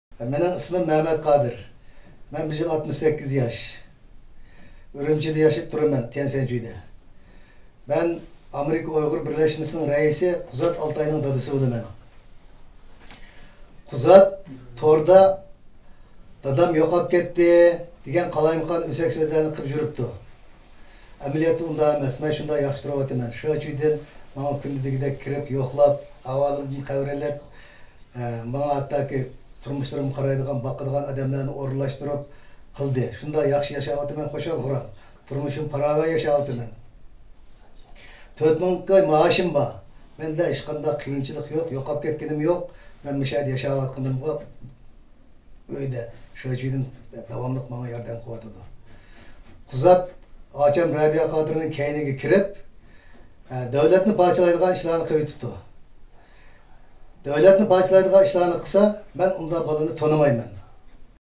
StillNoInfo The alleged missing Maimaiti Kadeer has something to say to his son
Recently,some people claimed on social media that they lost contact of their “relatives” and “friends”. For that, these “relatives”and “friends” being sought took the initiative to post videos to refute the rumor and tell the public that what those people claimed is a complete rumor and they actually have a good life.